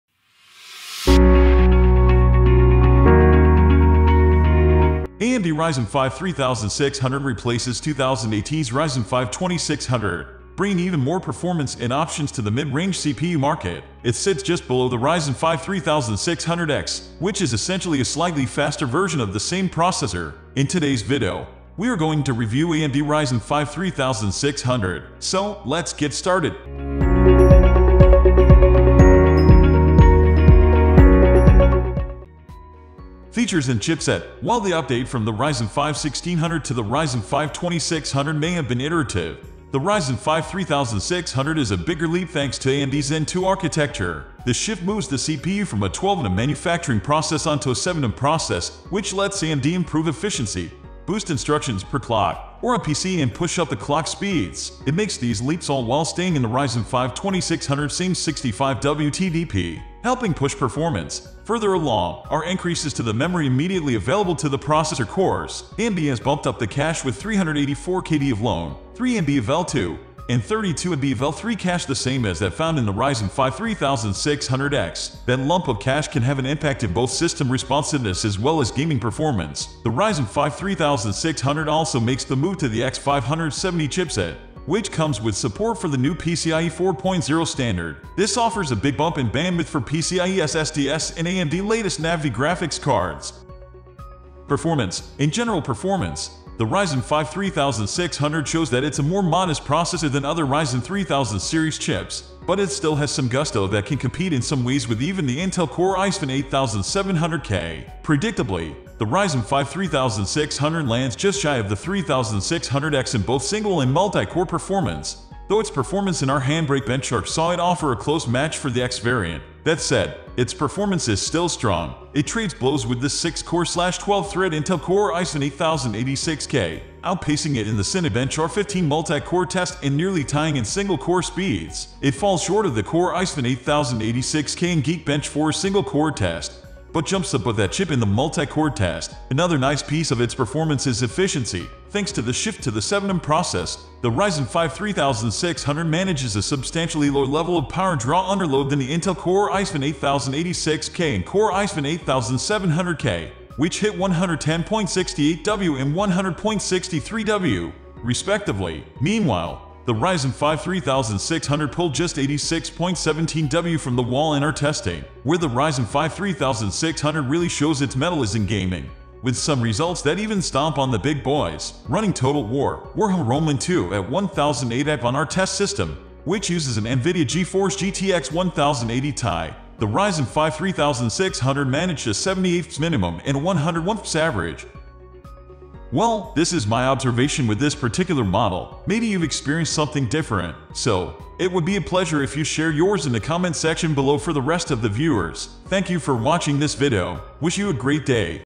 AMD Ryzen 5 3600 Review: Is it the Best Budget Gaming CPU?
AMD Ryzen 5 3600 Gaming CPU - The AMD Ryzen 5 3600 has been one of the most popular CPUs in recent years for gamers on a budget, and for good reason. With its impressive 6-core/12-thread configuration and high clock speeds, the Ryzen 5 3600 delivers excellent performance in gaming, as well as productivity tasks. In this video, we'll be reviewing the Ryzen 5 3600, going over its specs, benchmark results, and overall performance.

We'll be testing the Ryzen 5 3600 on a variety of games, including some of the most popular titles on the market, to see how it stacks up against other CPUs in its price range. We'll also be discussing the CPU's thermal performance, power consumption, and compatibility with various motherboards.

Overall, we'll give you a comprehensive overview of the AMD Ryzen 5 3600 and help you decide if it's the right choice for your gaming PC build.